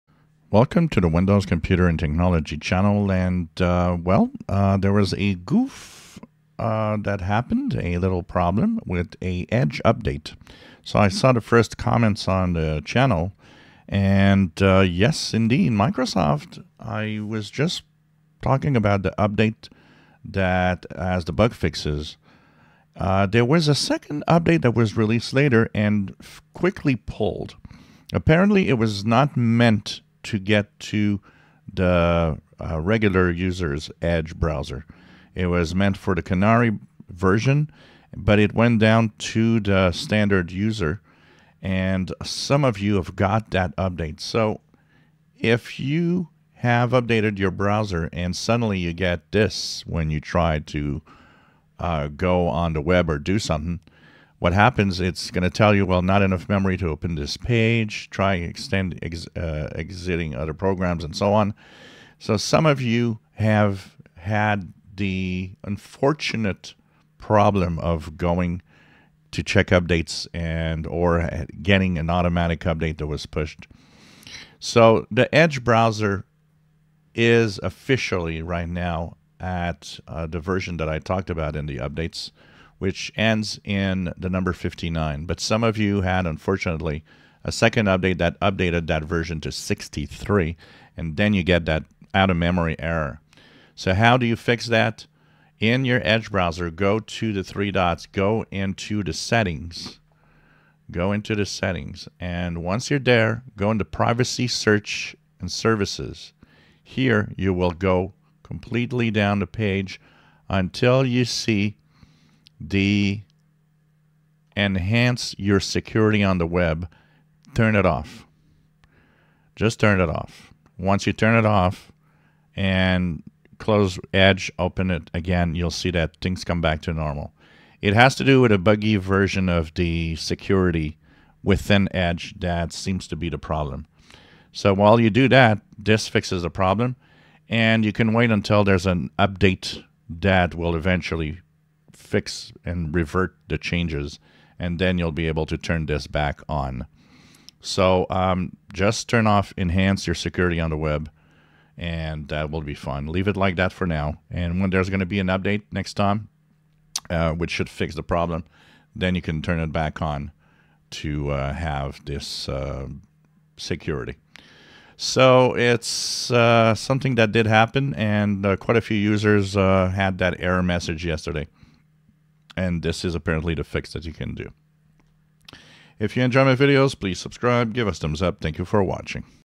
FIX Microsoft EDGE Out Of Memory error message after latest update
Microsoft accidentally pushed a buggy update for EDGE, you can fix it by turning off the web protection security as shown in the video